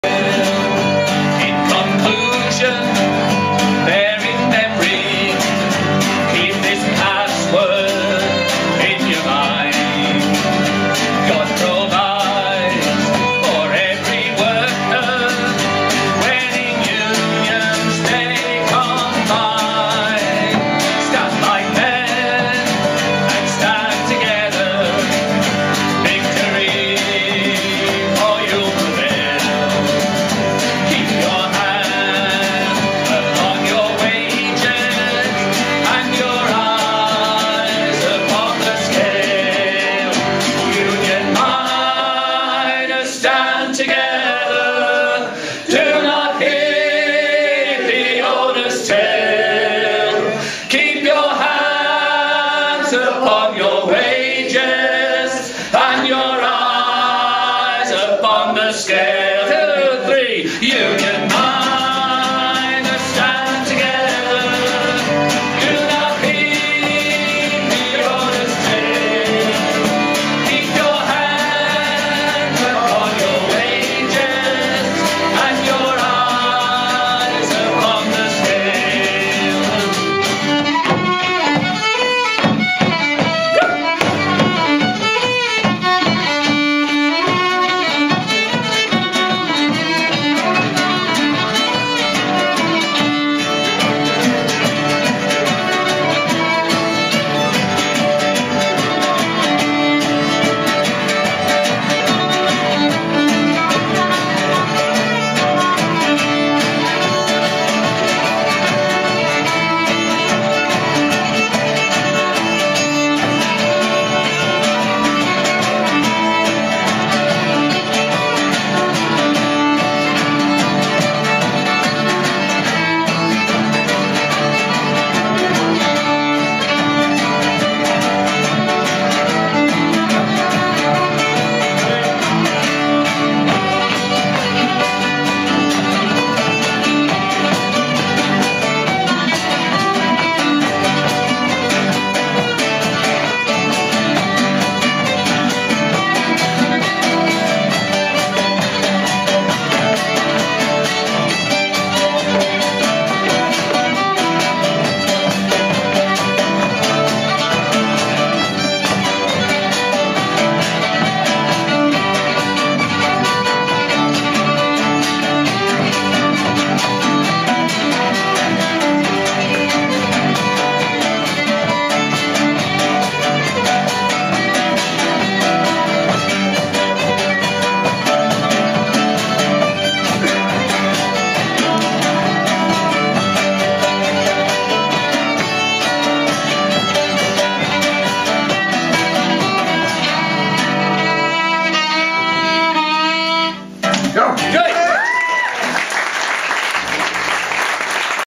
'North Road' at Darlington Folk Club 2:5:13
'North Road' (left to right) Peter French, Tony Martin, George Armstrong, Bob Tuddenham - 'unplugged' at Darlington Folk Club 2/5/13